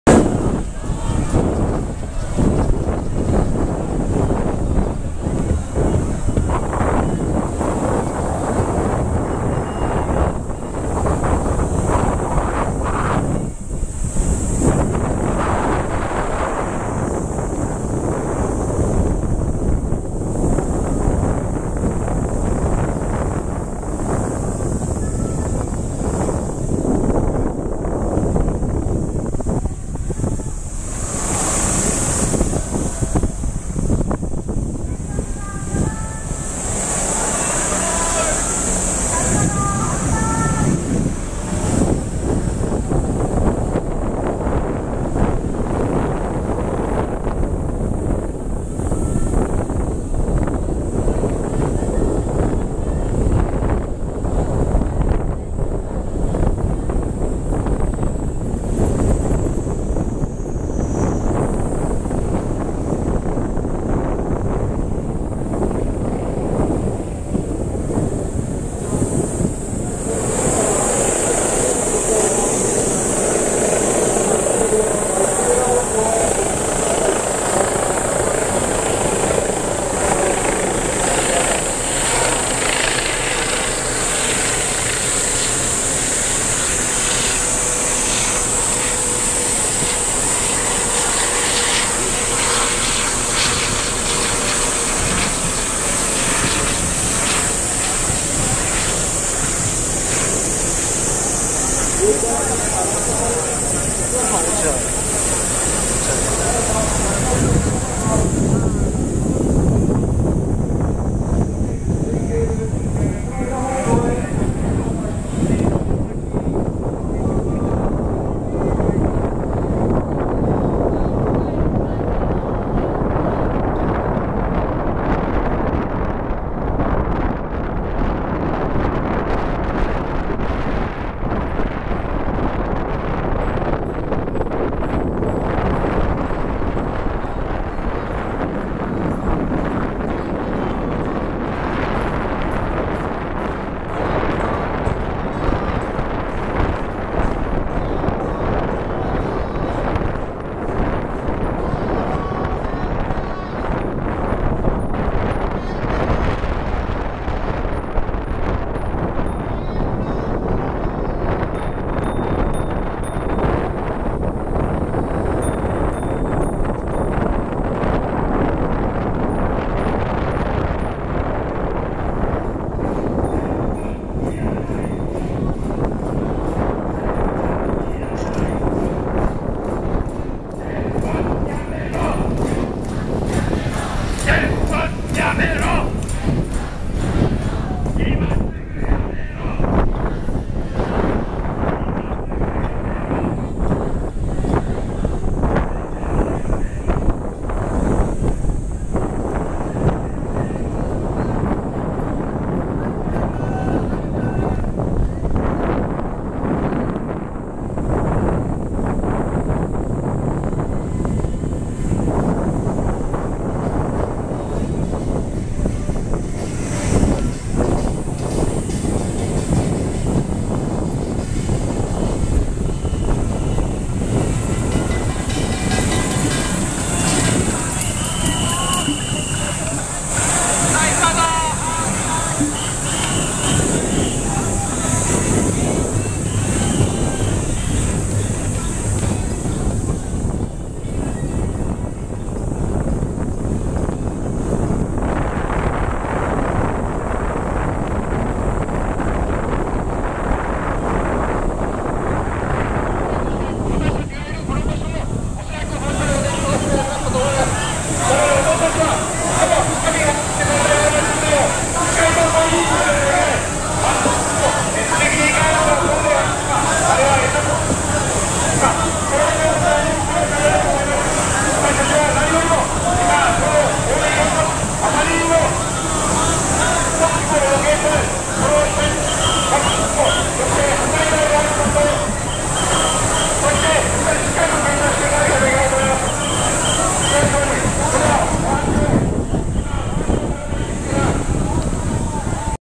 7.29 脱原発！ 国会大包囲デモ 車で国会正門から国会を1周してみました
7.29国会議事堂大包囲デモ　車道からの様子

※風の音がうるさくてすいません。